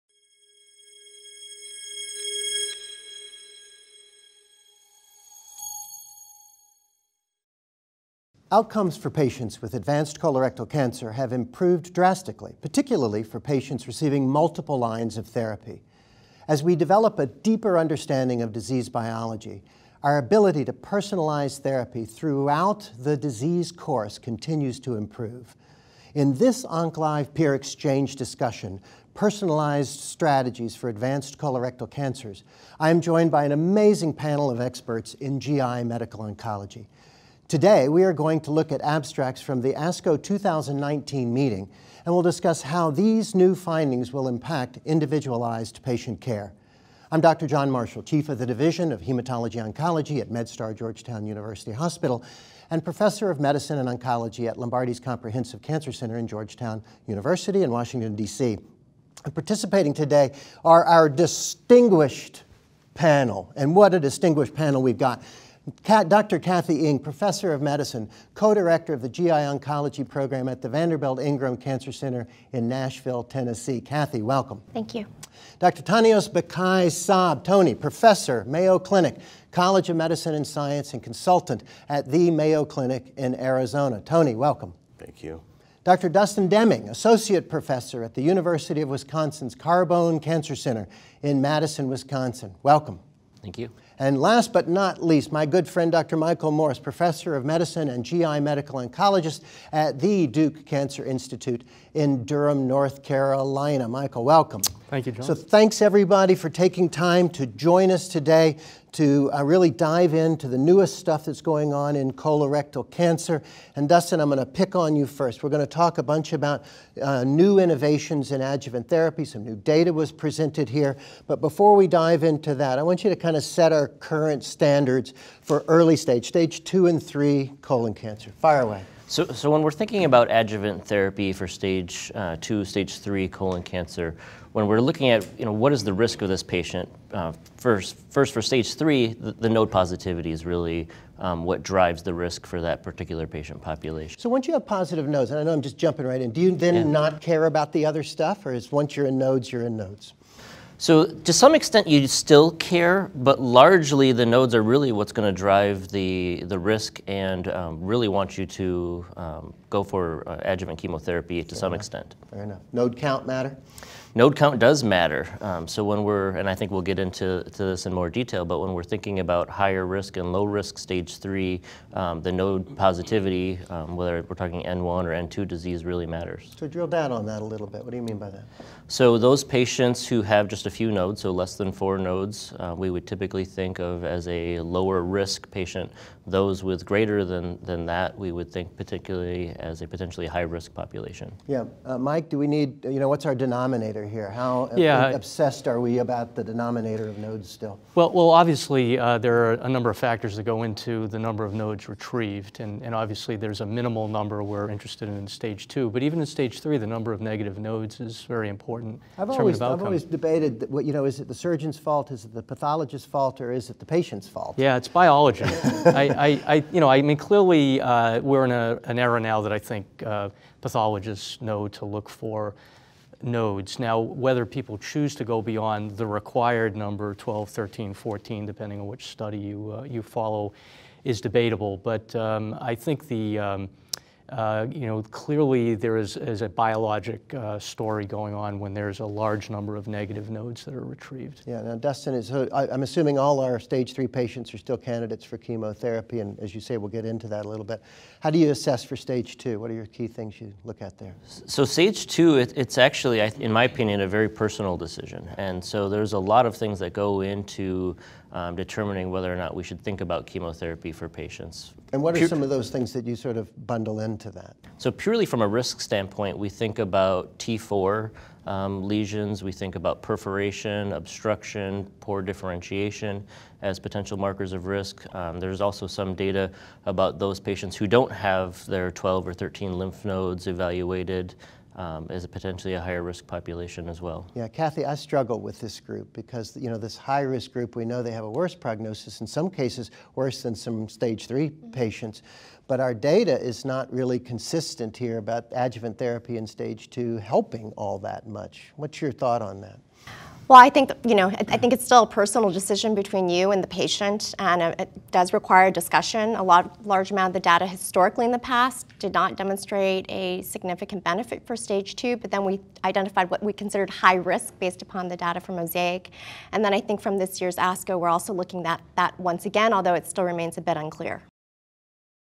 Adjuvant Therapy for Stage II and III Colon Cancer
Discuss risk for recurrence based on nodal status in patients with stage II and stage III colon cancer and personalized decision making surrounding adjuvant therapy.